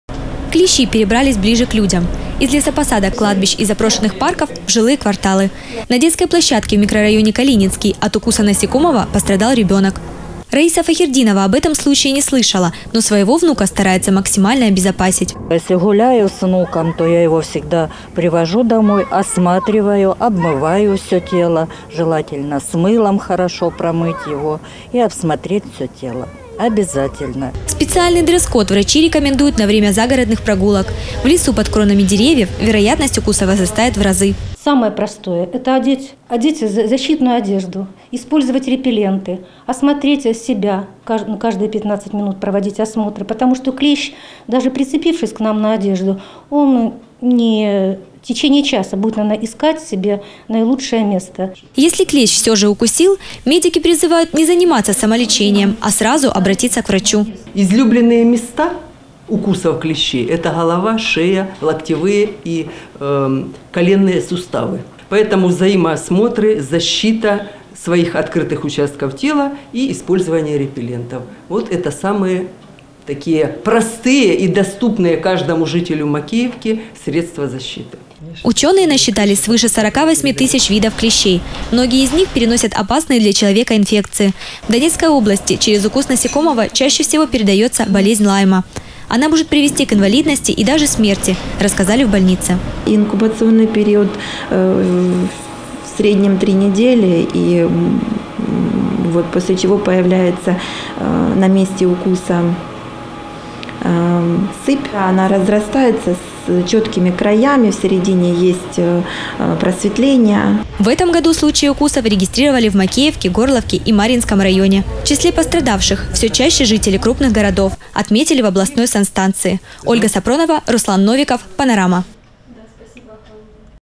Семеро пострадавших за две недели
Семеро пострадавших за две недели. В Макеевке клещи кусают людей. В большинстве случаев в больницы попадают дети. Обычно, пик активности насекомых приходится на конец мая. Но медики призывают население позаботиться о своей безопасности уже сегодня. Как это сделать, выясняли мои коллеги.